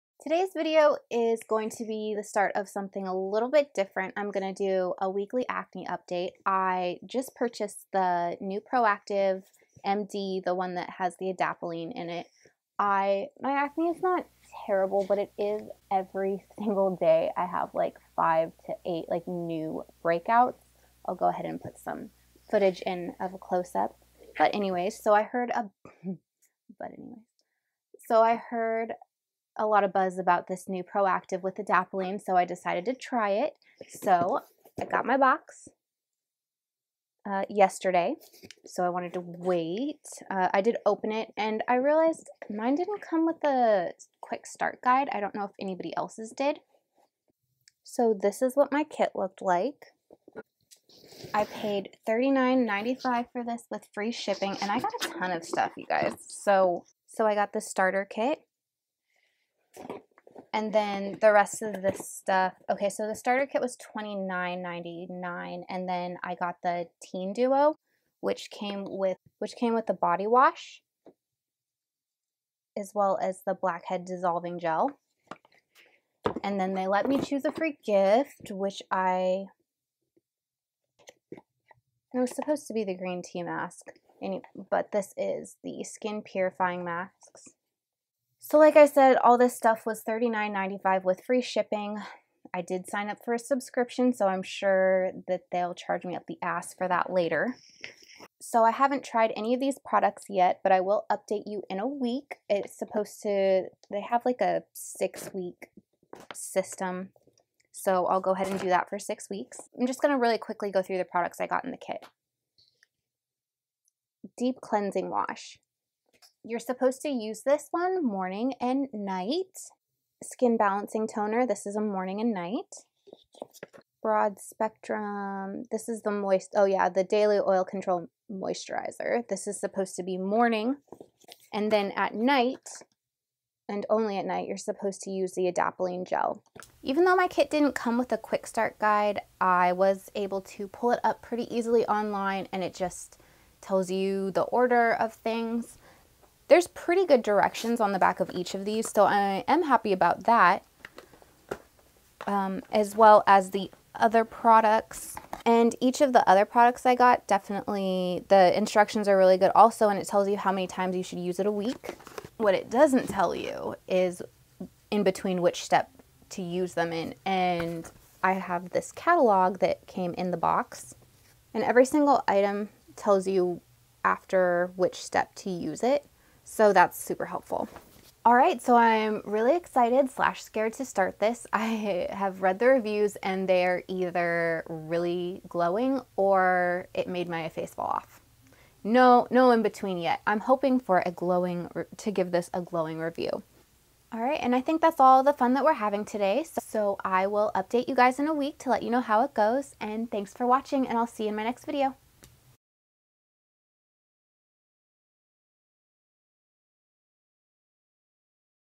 ProactivMD || Update#1 -Start and Unboxing
Hey Humans!
Ive been struggling with constant breakouts for the last year so I decided to give the new Proactiv MD a try.  Follow me for the next 6 weeks as I give weekly updates about how my skin is doing

Products Mentioned:
Proactiv MD Deep Cleansing Face Wash
Proactiv MD Balancing Toner
Proactiv MD Daily Oil Control Moisturizer with SPF 30
Proactiv MD Adapalene Gel
Proactiv Blackhead Dissolving Gel
Proactiv Skin Purifying Mask
Proactiv Deep Cleaning Wash- for body